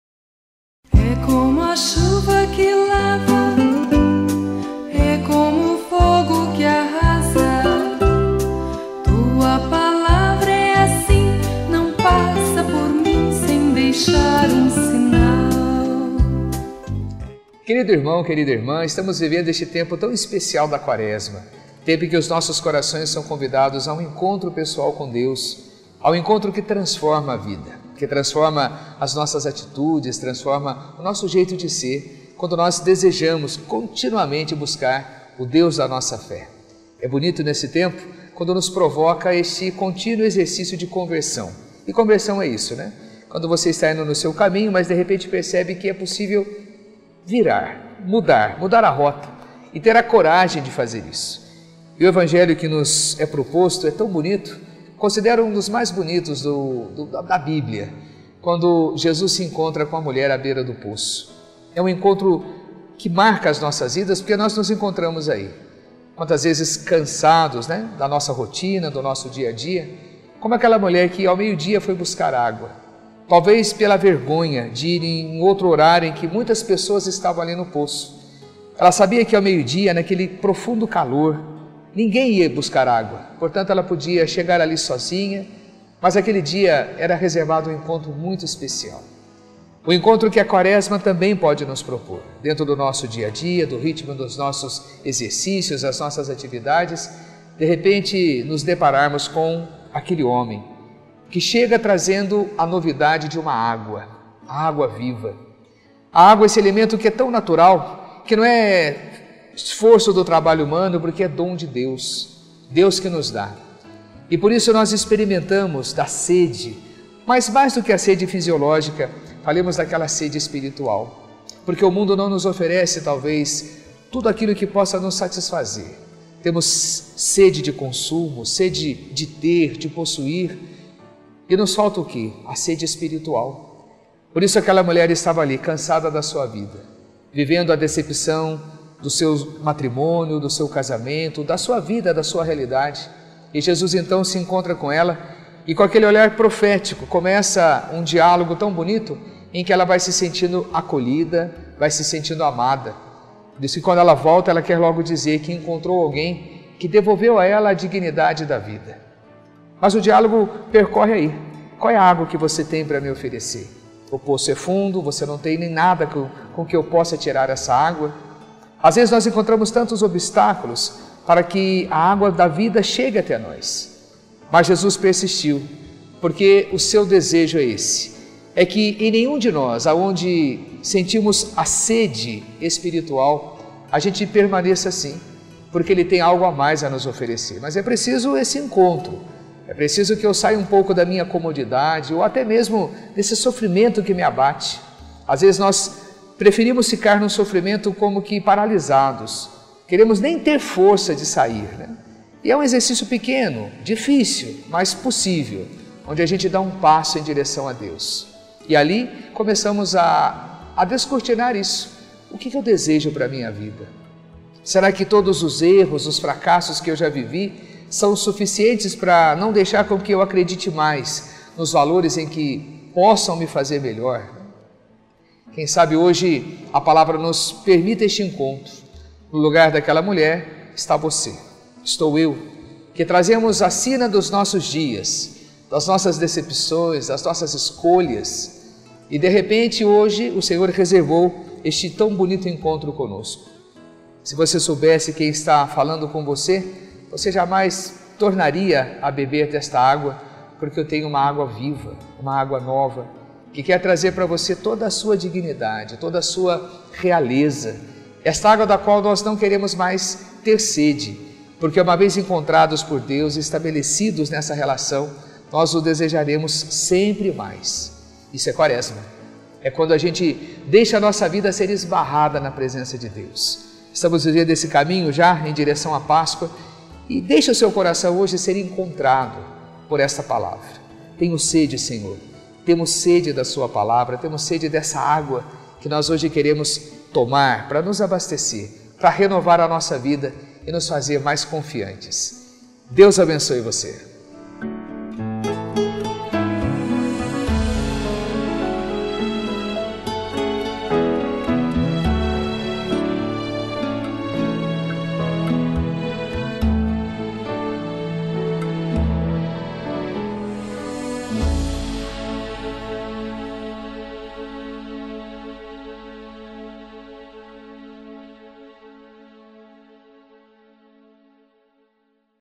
Viver a Palavra | Água Viva
No 3º domingo da quaresma contemplamos o Evangelho de João 4,5-42, onde Jesus encontra a mulher junto ao poço. É este encontro pessoal que devemos buscar na quaresma, converter nosso coração e buscar a água viva que vem de Deus. Pe. Silvio César, sdb, nos ajuda a refletir sobre este importante caminho quaresmal.